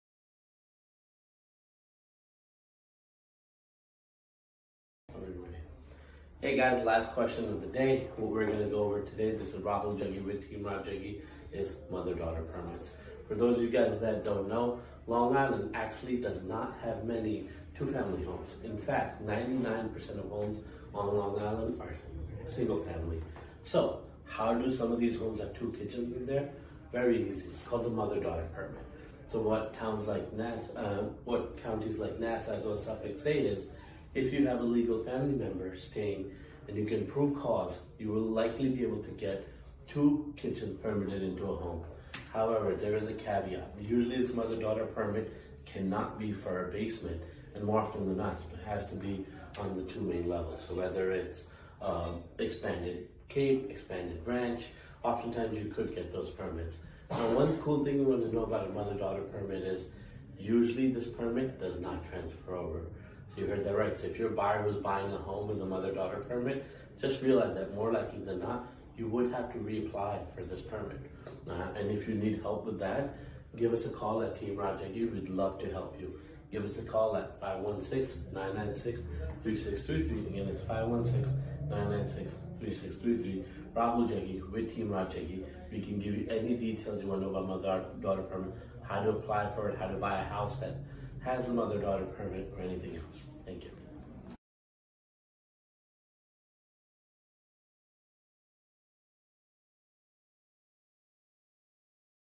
🏡 Did you know that Long Island barely has any two family homes?
🏡 Did you know that Long Island barely has any two family homes? About 90% of the homes in Long Island are single family owned. So, how do some of these homes have two kitchens? It’s because of the mother-daughter’s permit. Watch the video further to know more about this permit.